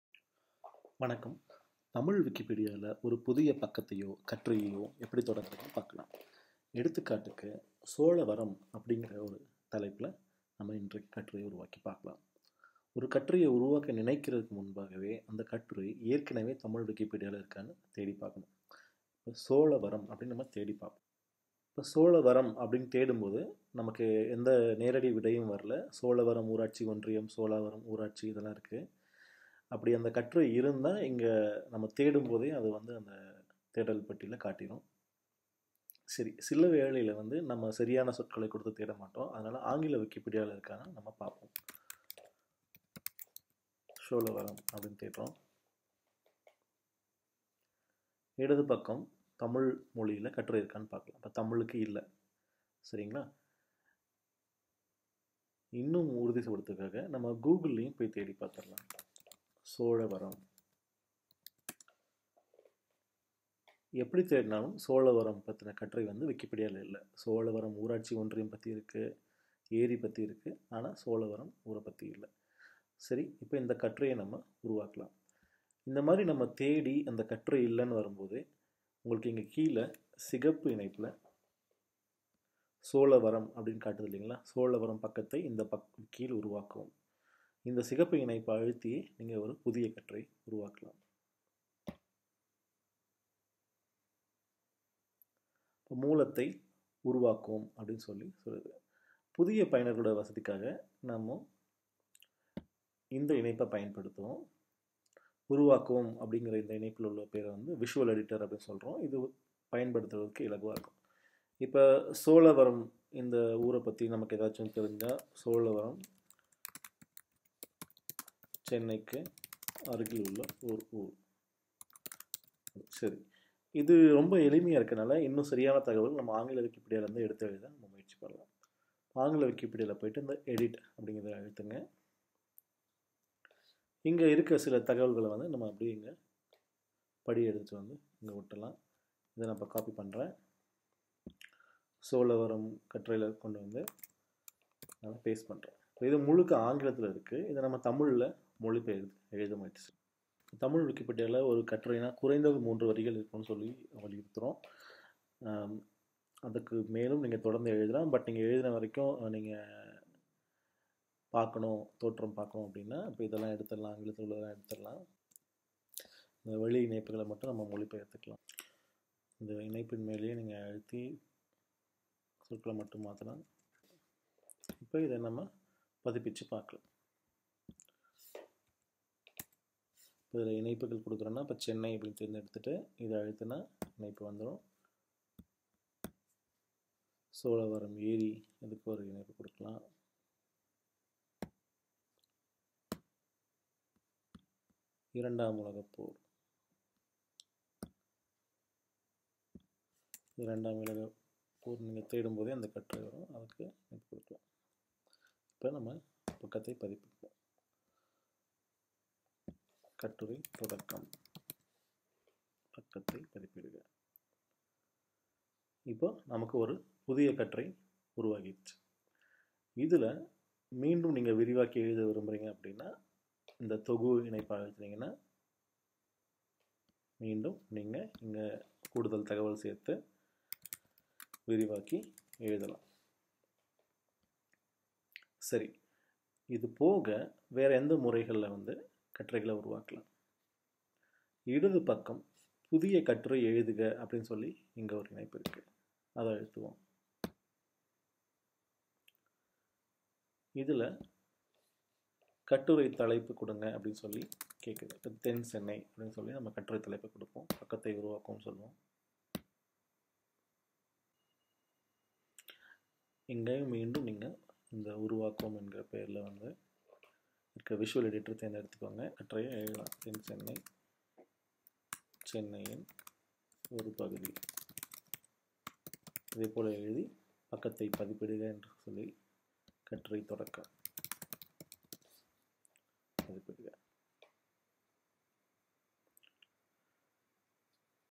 தமிழ் விக்கிப்பீடியாவில் புதிய கட்டுரைகளை உருவாக்குவது எப்படி?
தமிழ் விக்கிப்பீடியாவில் புதிய கட்டுரைகளை உருவாக்குவது எப்படி என்பதை விளக்கும் படம்